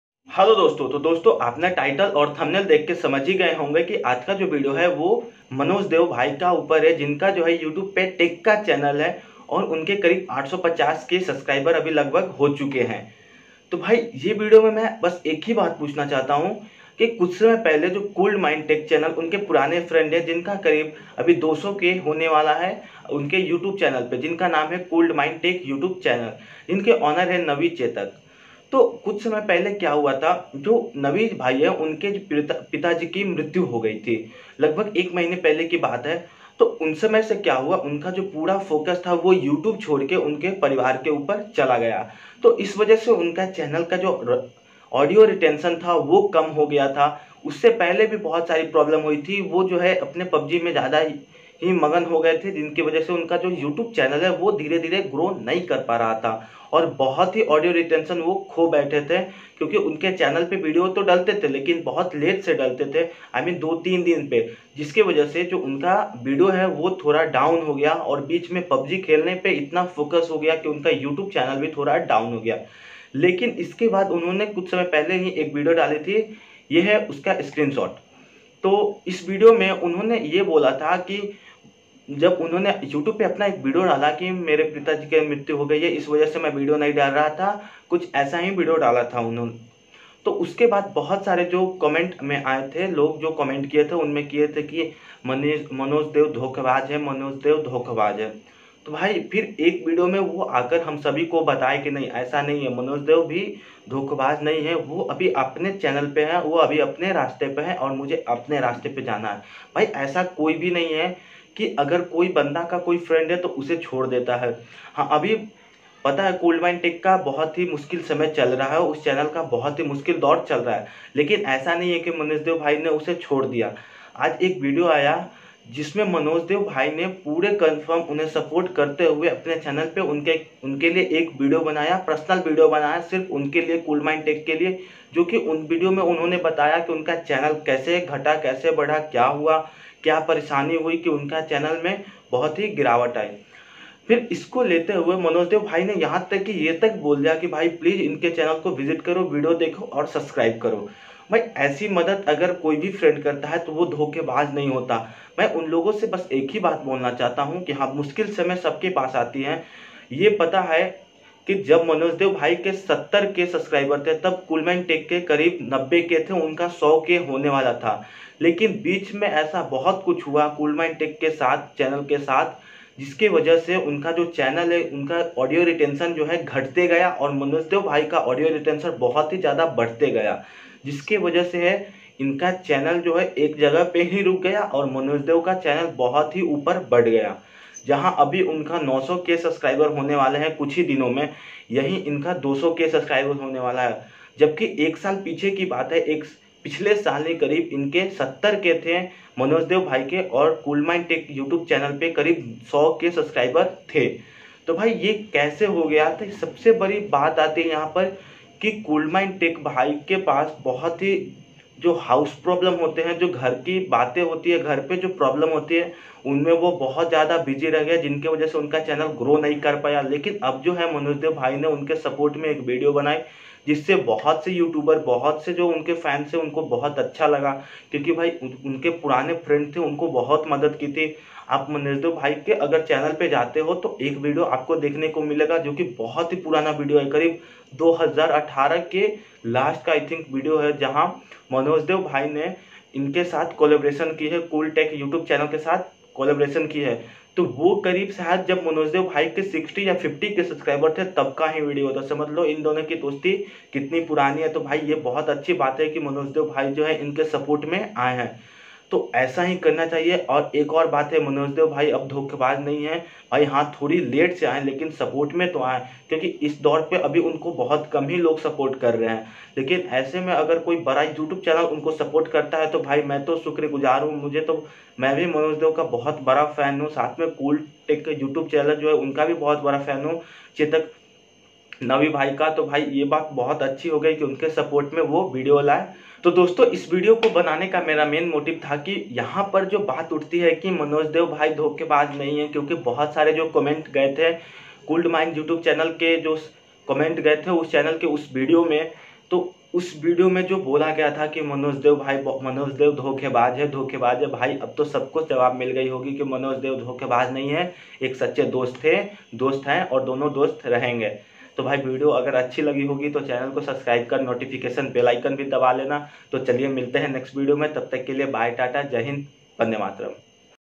क्या Manoj dey धोखेबाज है | Cool Mind Tech
Hlo Dosto aj ki ye video bhut hi kamal ki video is video m aapko pata chal jayega msnoj dey kaise insaan hai . aap video ko jarur dekhe.

MY GEARS-:

MY FIRST PHONE
MY MIC-:
MY RINGLIGHT
MY SECOND PHONE -:
CAMERA USED